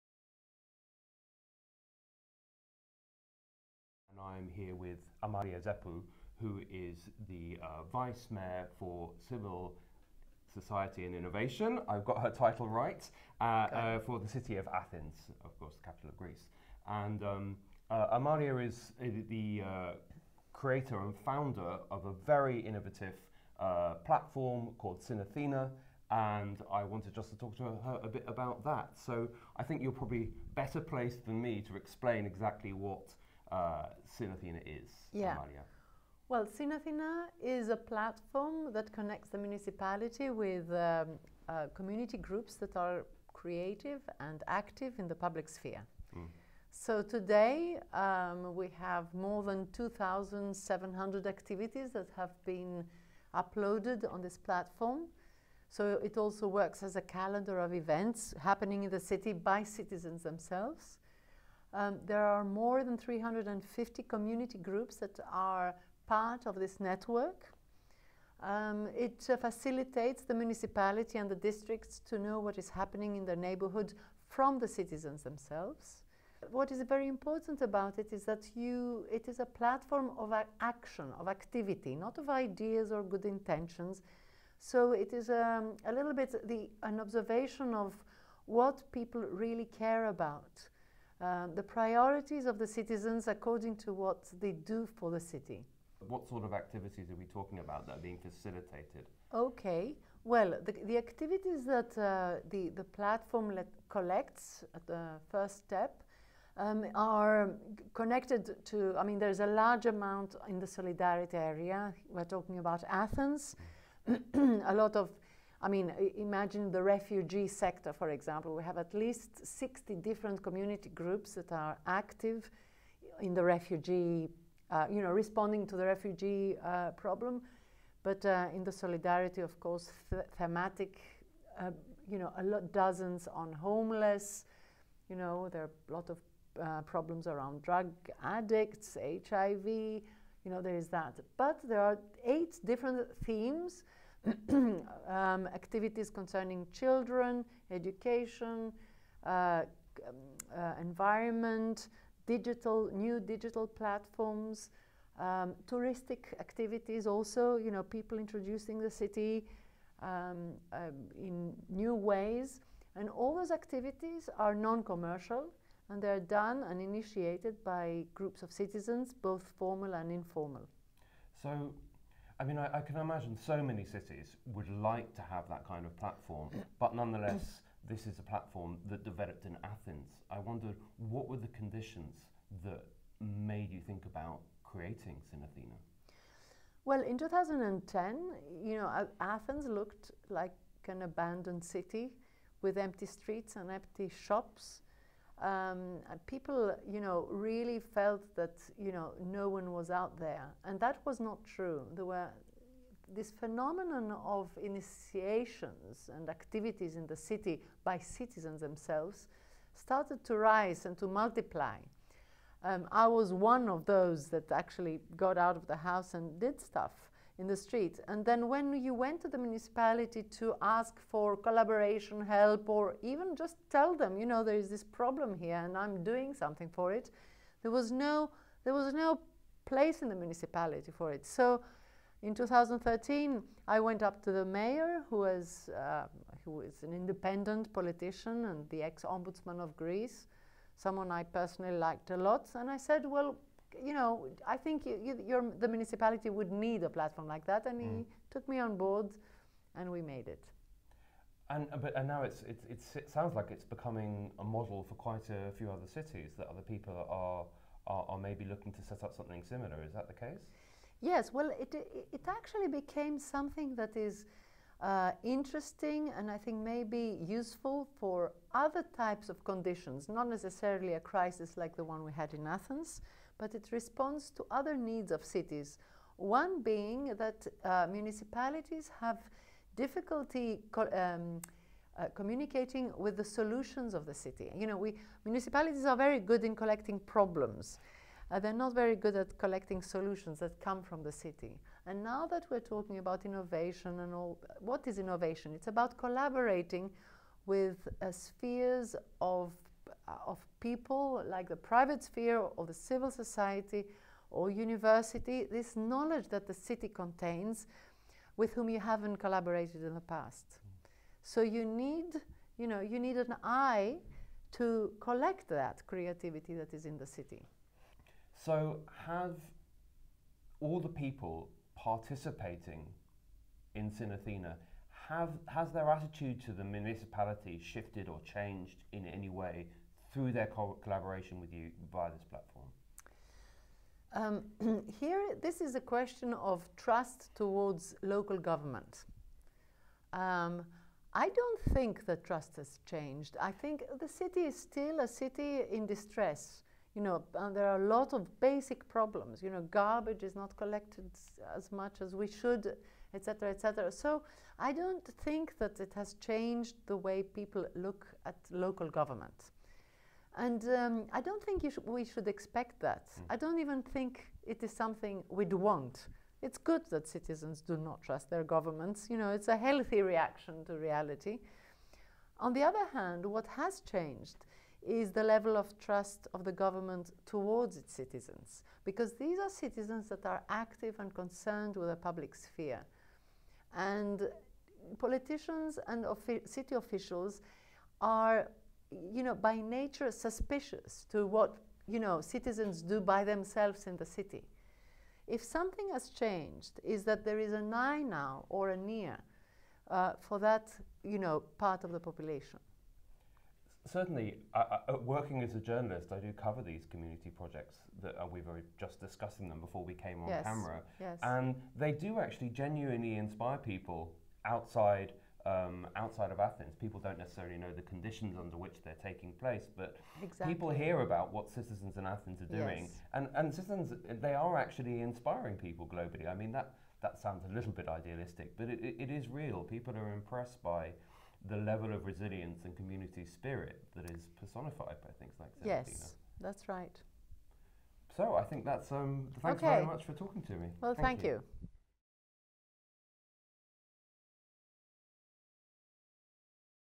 Connecting Communities in Athens
A conversation with Amalia Zepou, Athens’ Vice Mayor for Civil Society and Innovation.